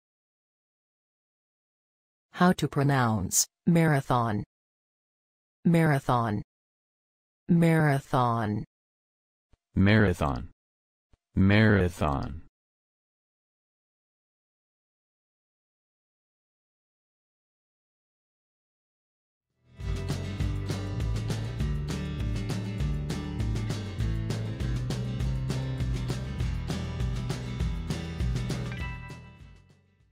Marathon pronunciation
How to pronounce Marathon in English? Listen with us.
What is the correct pronunciation of the word Marathon in everyday English? Speak as the Americans.
How to write the word Marathon? Watch and learn.

Learn how to pronounce Marathon.